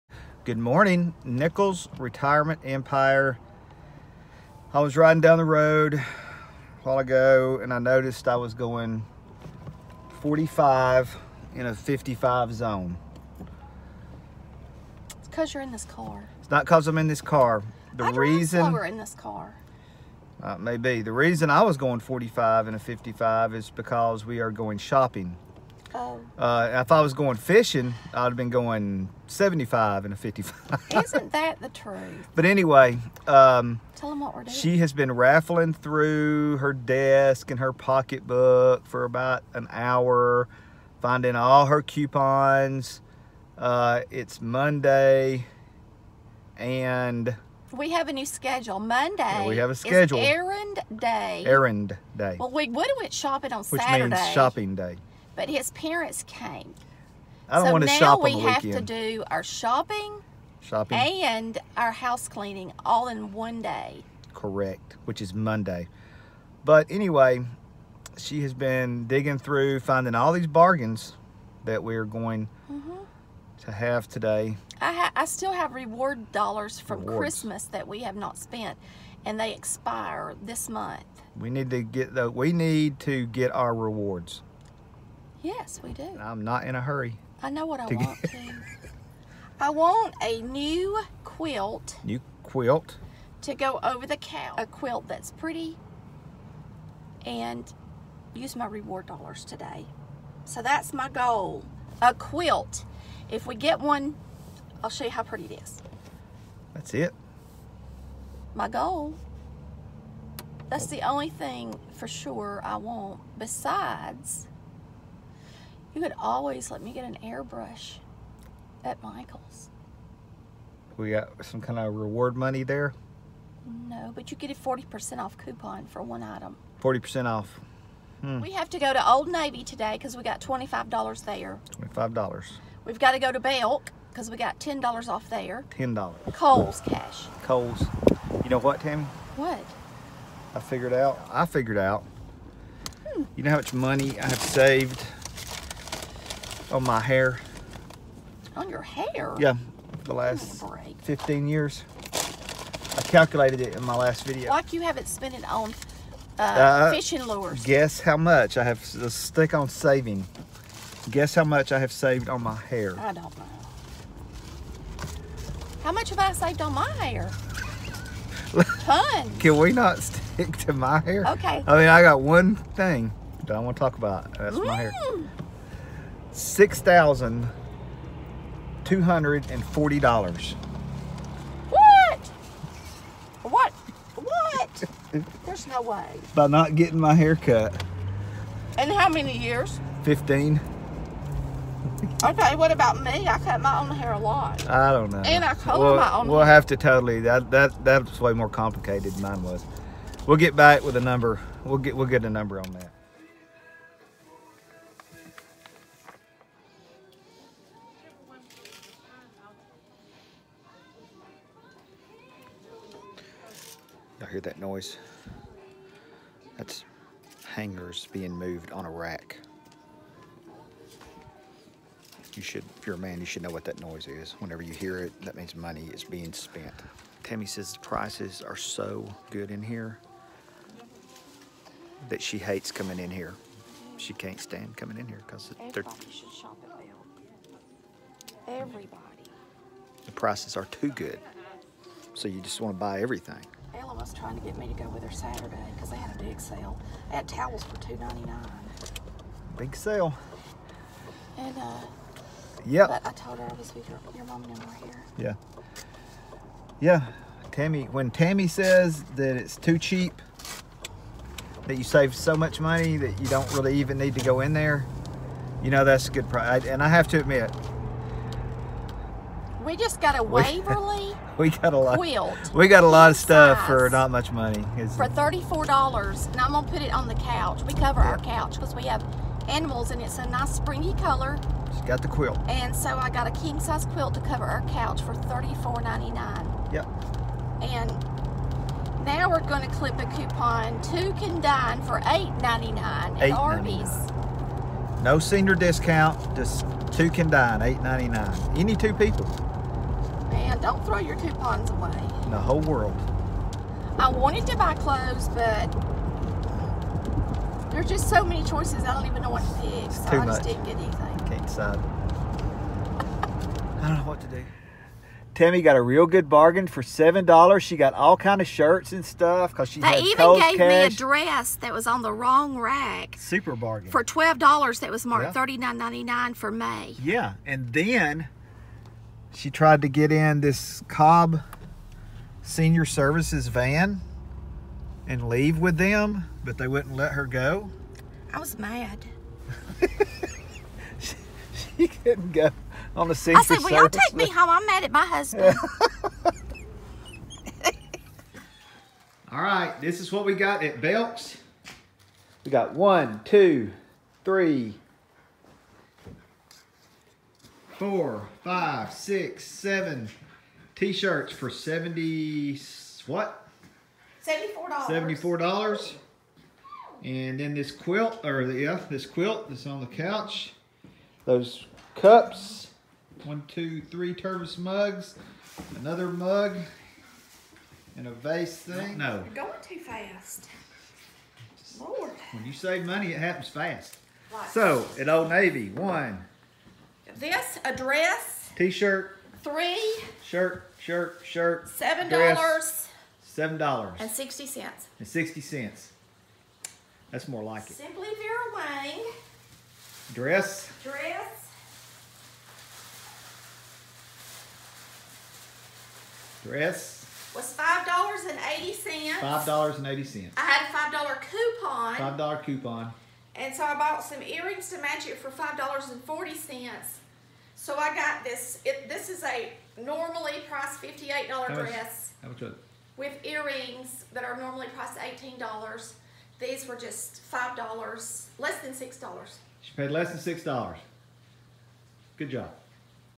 Tammy Hates This Store-Nichols Retirement Empire
Tammy Hates This Store-Nichols Retirement Empire. Mondays are errand days. Tammy had some store cash to spend! We went to Old Navy, Kohls and Belks. See which store had such good prices that Tammy said she hated going there!